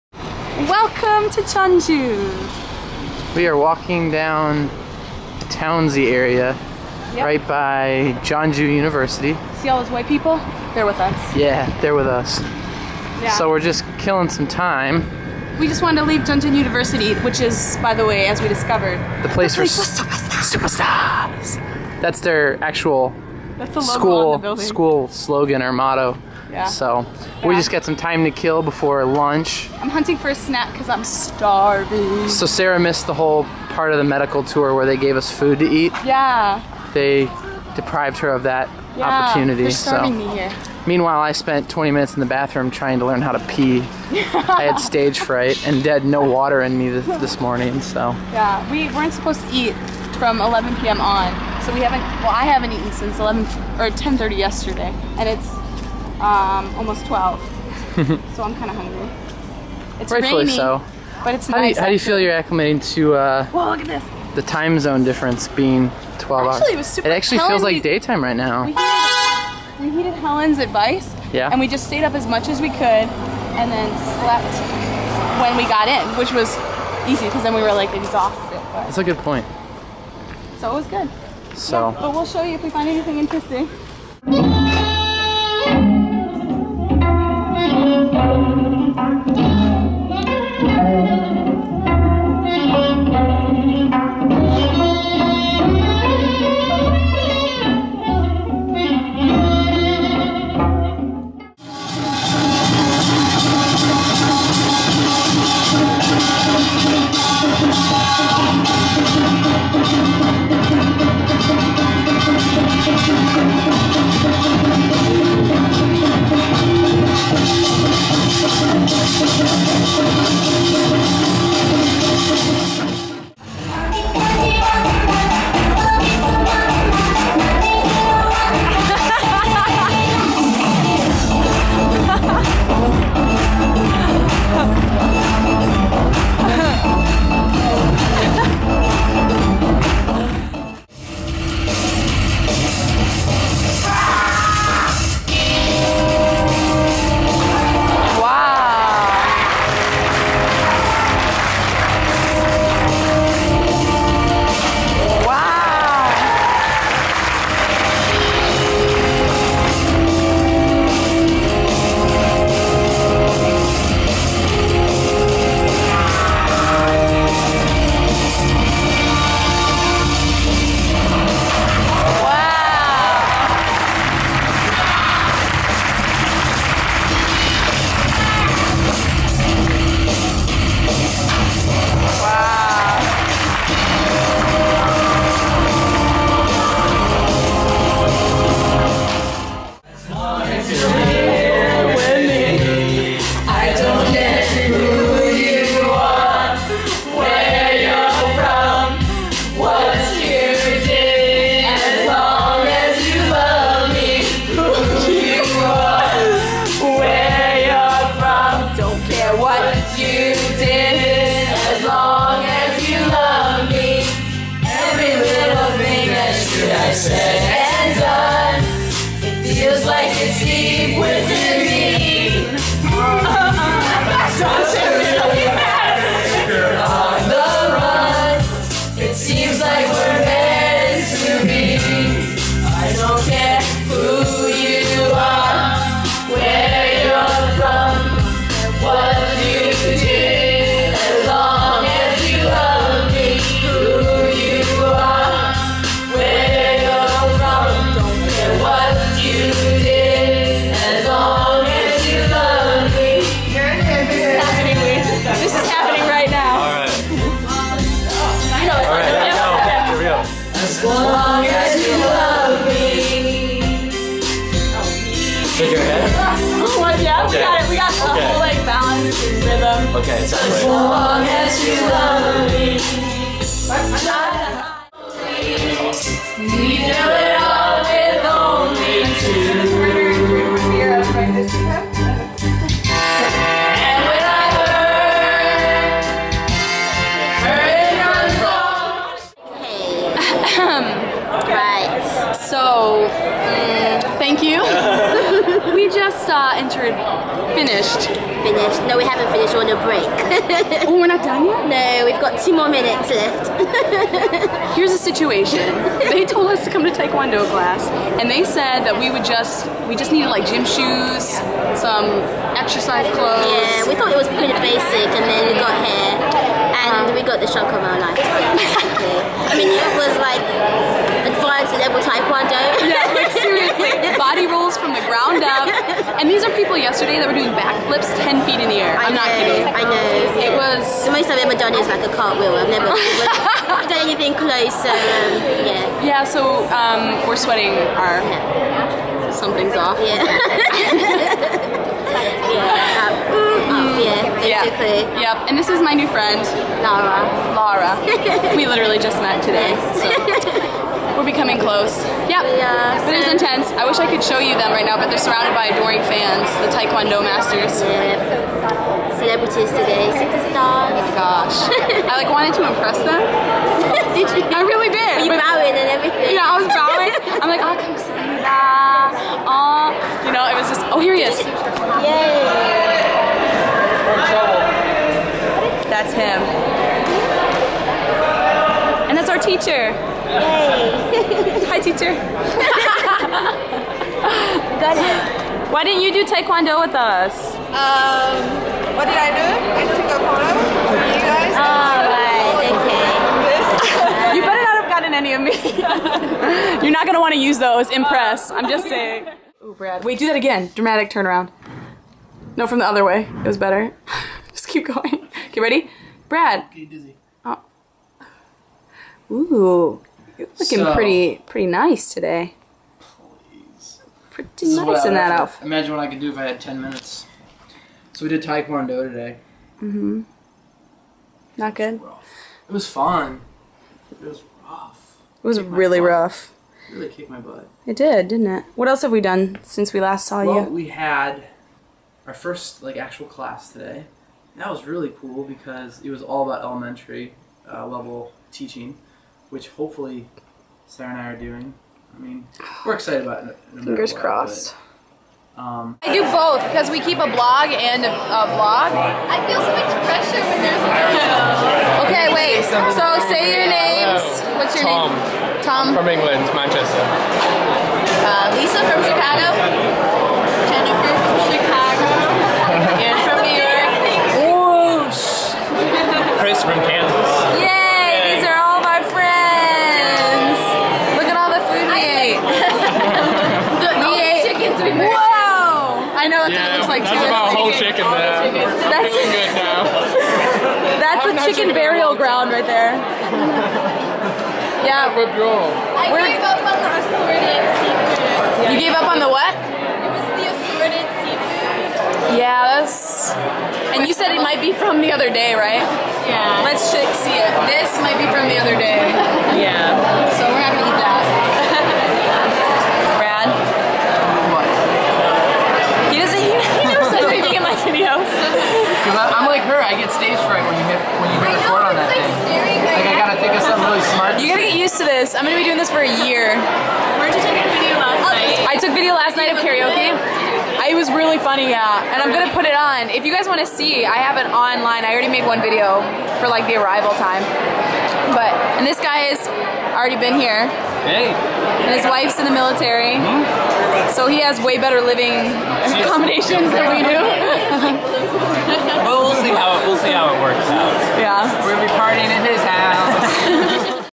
Tae kwon Do Kicks Your Face Off!
Days 2 and 3 of our adventure in South Korea consisted of Fan Dancing, Crazy Tae kwon Do, Karaoke, and getting our butts kicked by the Tae Kwon Do masters doing backflips the day before.  Good times.